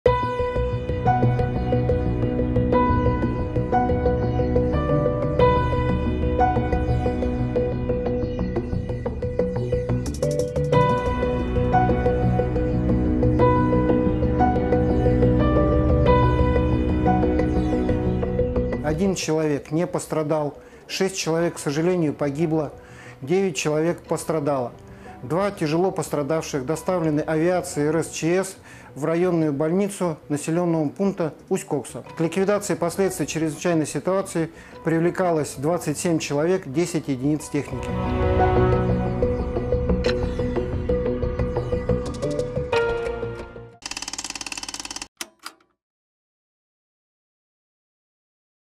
Russian Mi-8 Helicopter Crashes In Siberia, Killing 6
A helicopter crashed in Russia's Siberia on Thursday, killing six of those on board and injuring seven, Russian emergency officials reported. The Mi-8 helicopter in the Altai Republic in southern Siberia caught fire as it was landing and brushed against a power line, the Altai branch of Russia's Emergency Ministry said.

n18oc_world      n18oc_crux